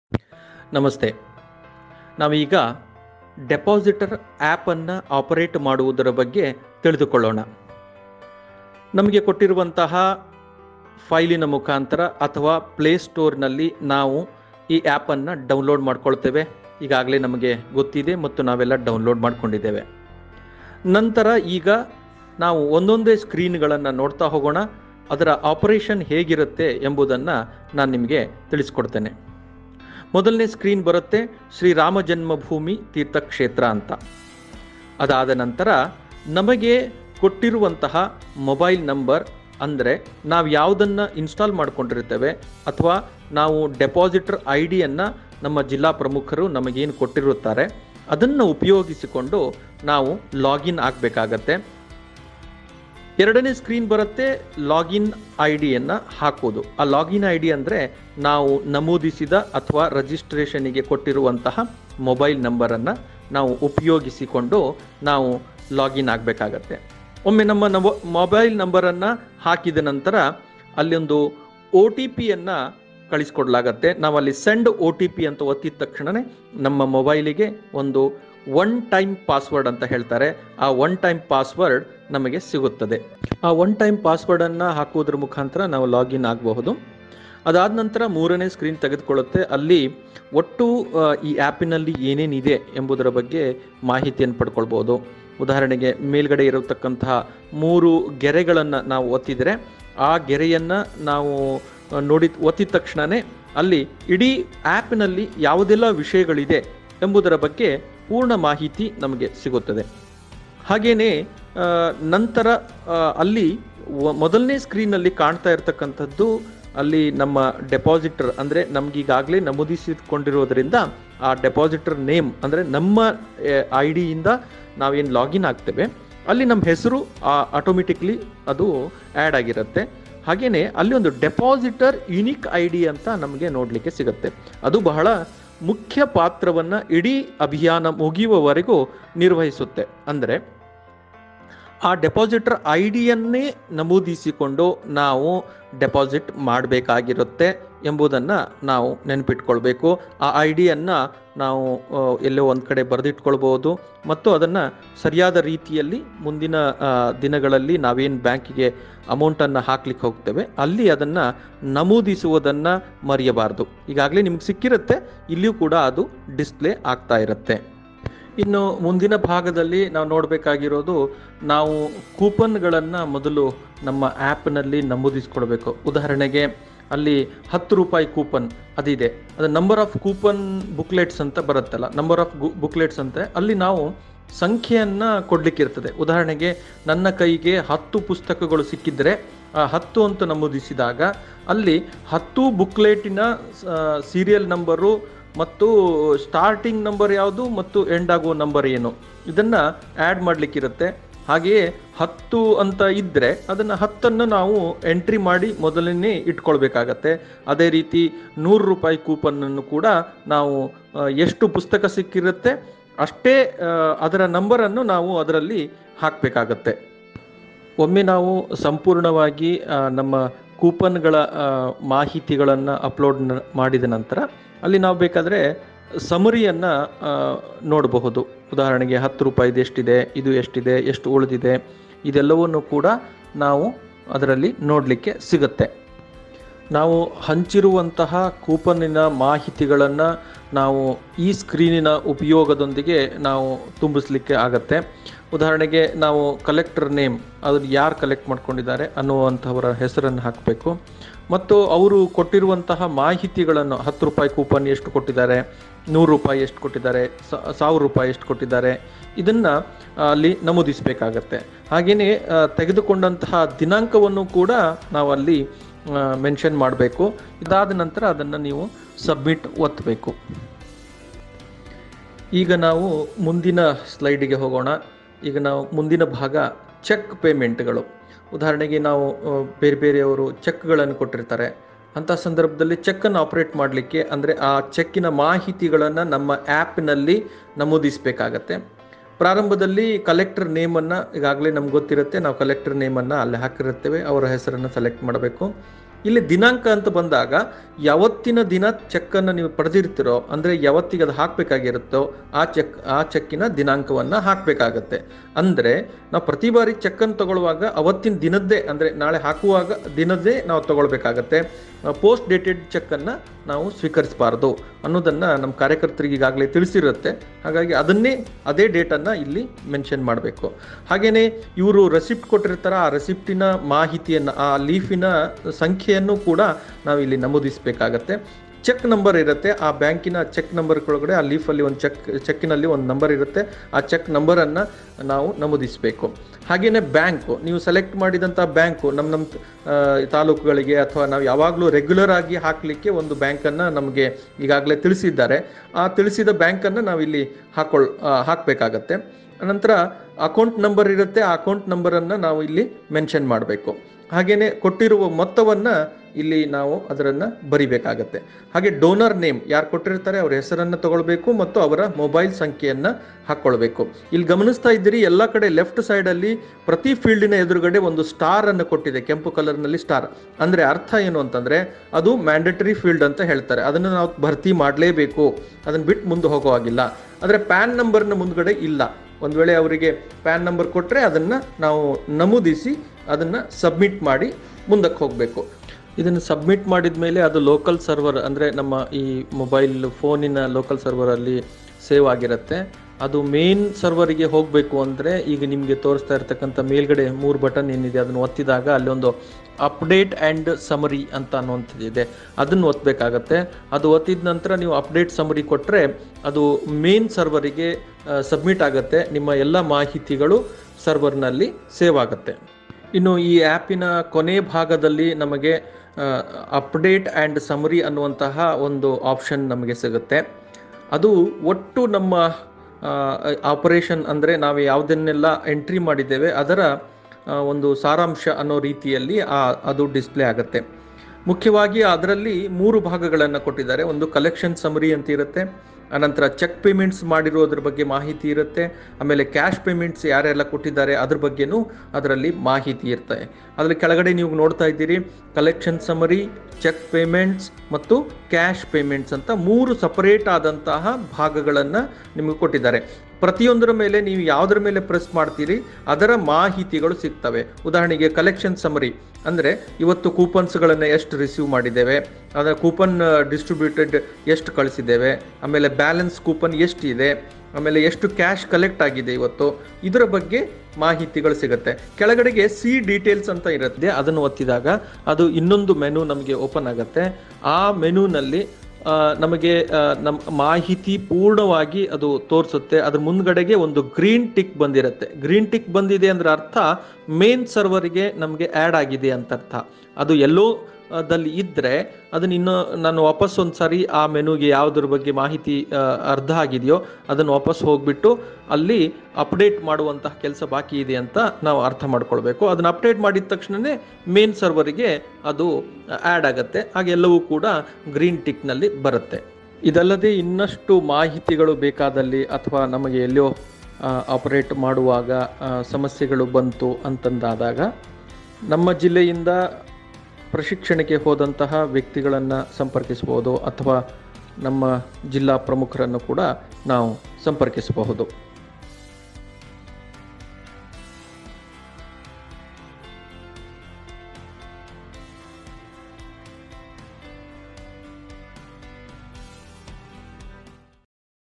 ಶ್ರೀರಾಮ ಜನ್ಮಭೂಮಿ ನಿಧಿ ಸಮರ್ಪಣಾ ಅಭಿಯಾನದಲ್ಲಿ Depositor App Demo
ಶ್ರೀರಾಮ ಜನ್ಮಭೂಮಿ ನಿಧಿ ಸಮರ್ಪಣಾ ಅಭಿಯಾನದಲ್ಲಿ Depositor App ಉಪಯೋಗಿಸುವುದರ ಬಗ್ಗೆ ಮಾಹಿತಿ